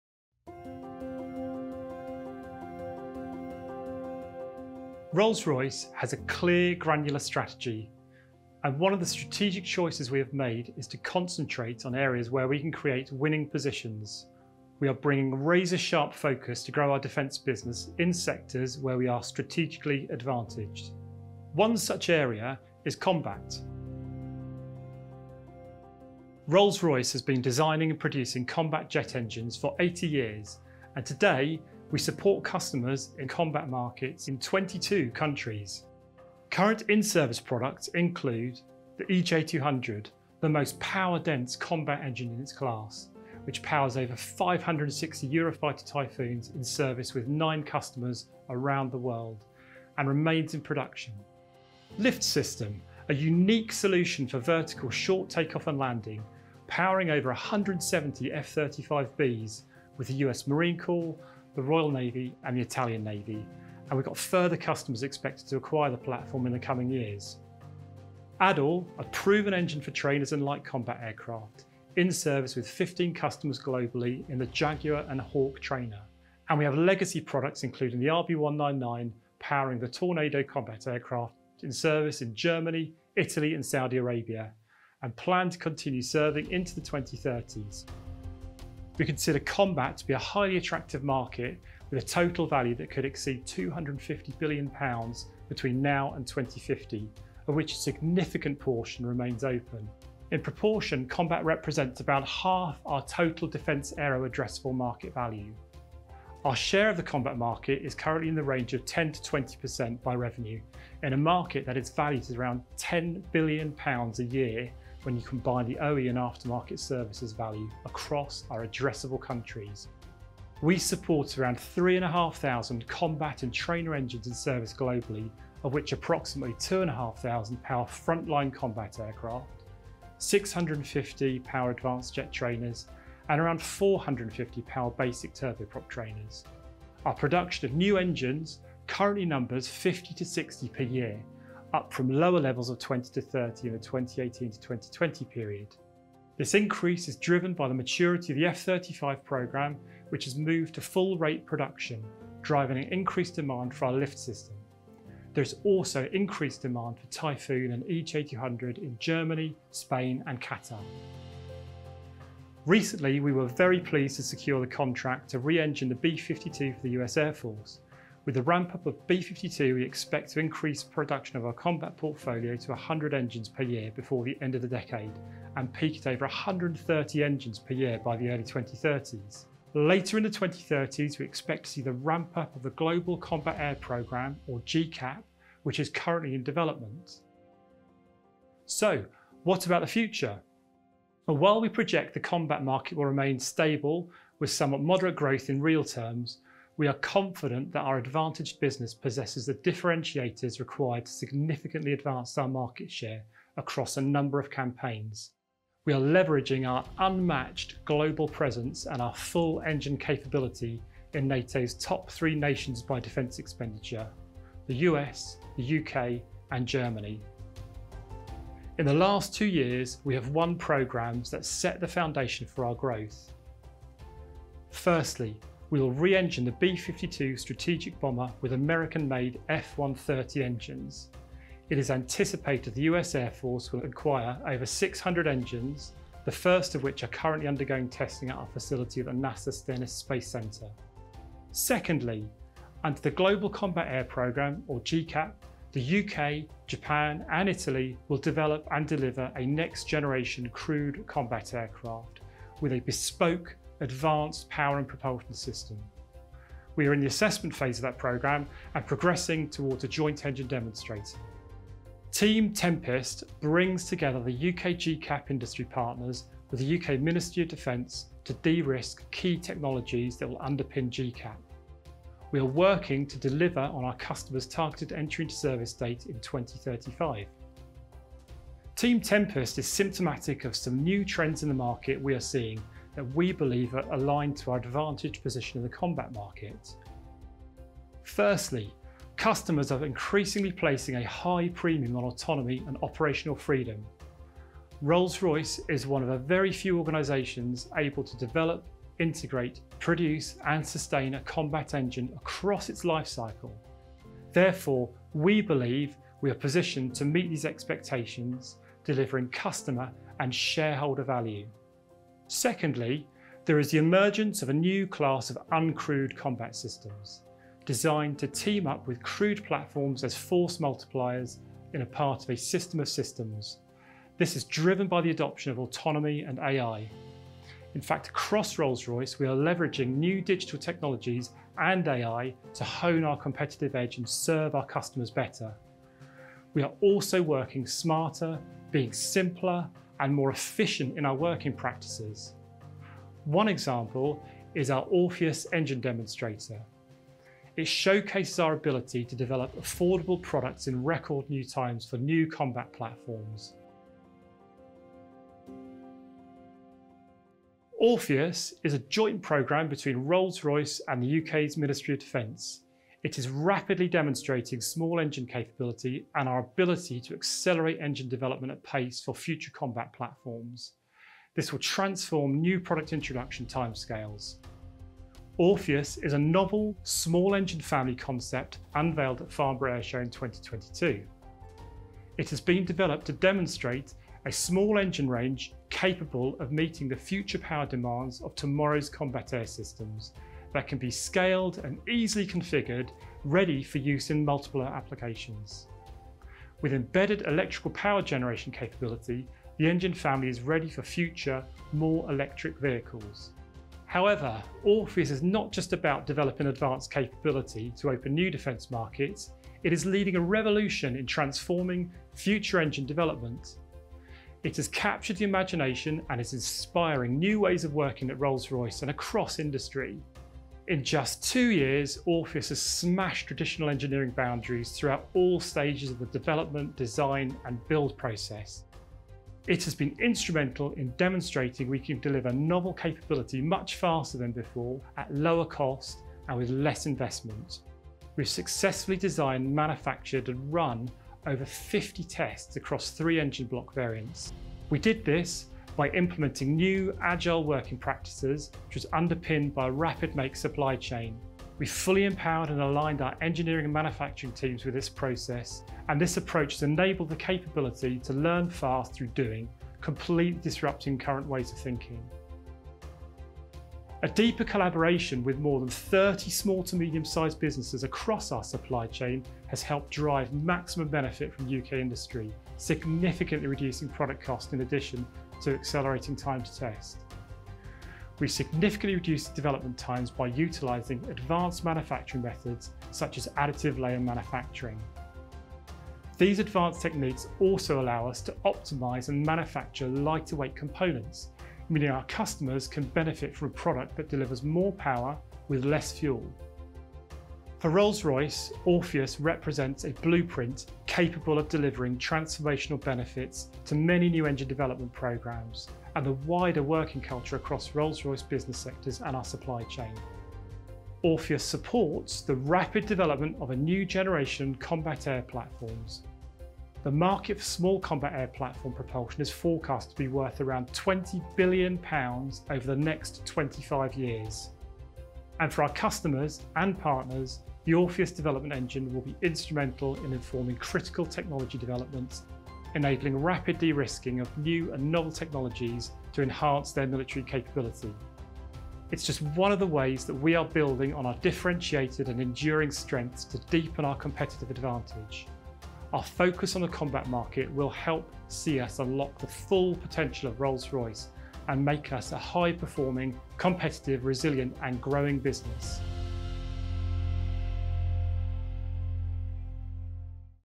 Rolls-Royce | Grow Combat and Orpheus
Rolls-Royce has been designing and producing combat jet engines for 80 years. Today, we support customers in this market across 22 countries. Orpheus is a novel joint programme between Rolls-Royce and the UK Ministry of Defence, which demonstrates a small engine range that can meet the power demands of tomorrow’s combat air systems.